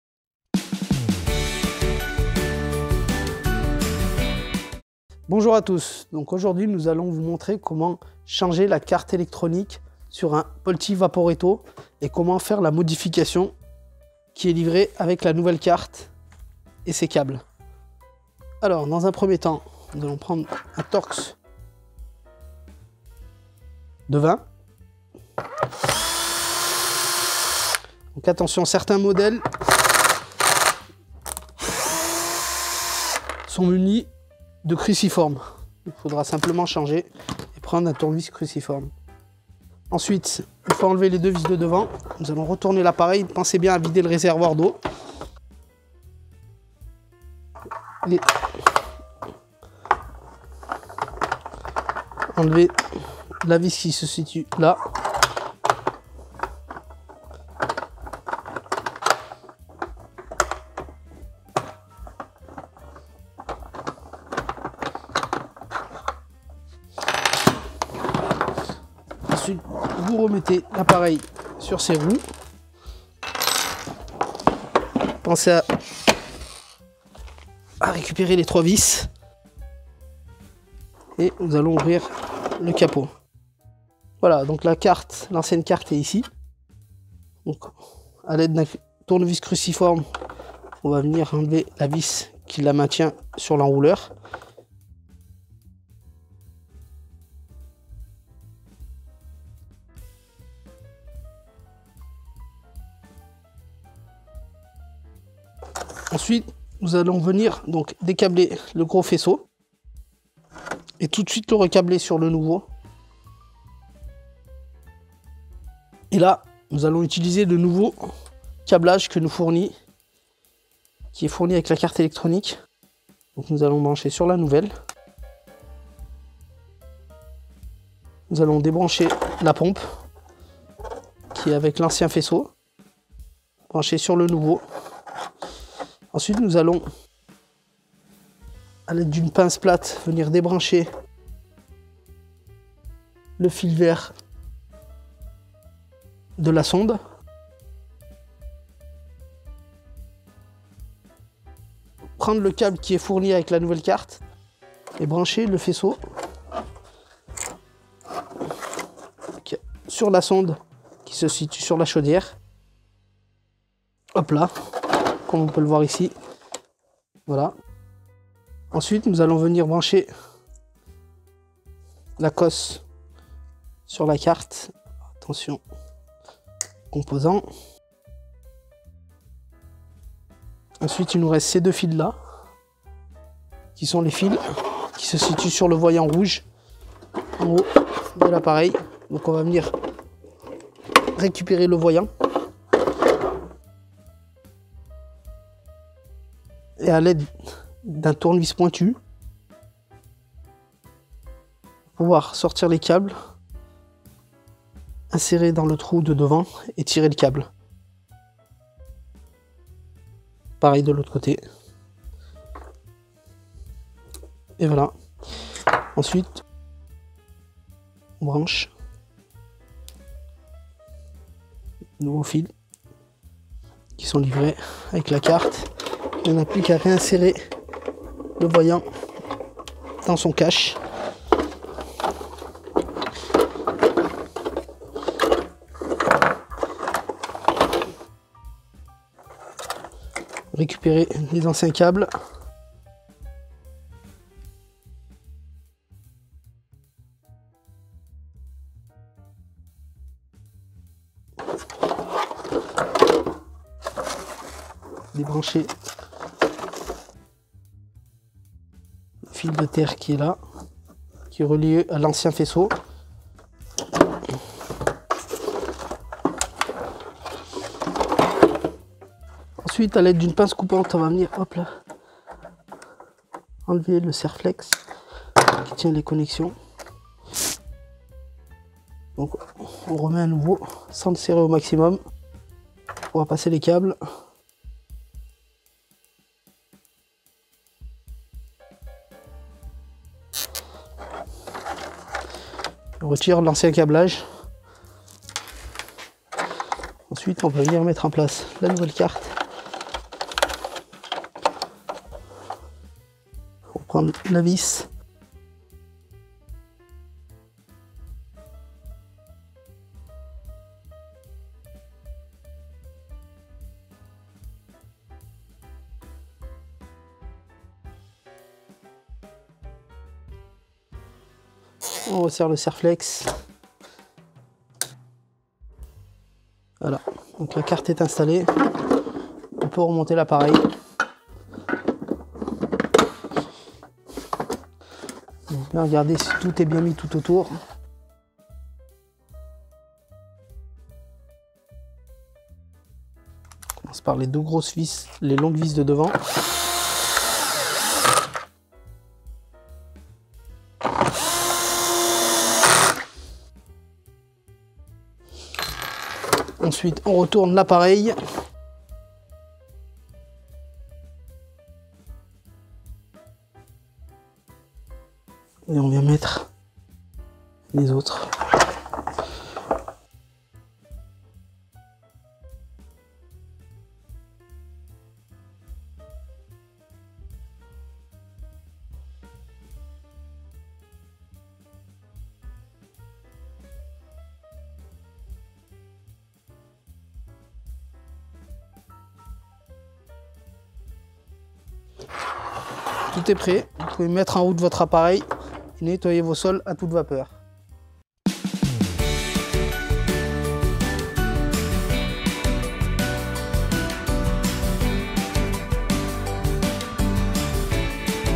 Comment remplacer la nouvelle carte électronique d'un Vaporetto Polti ?
Dans cette vidéo, apprenez comment remplacer la carte électronique de votre nettoyeur vapeur Polti Vaporetto. Ce tutoriel vous montrera pas à pas comment installer le nouveau kit de carte sur certains modèles de Vaporetto PTEU Polti.

🔧Commandez toutes les pièces qui vous manquent sur notre site en un clic ➡️

➡️Jetez un oeil au contenu de notre site : pièces détachées, articles, conseils...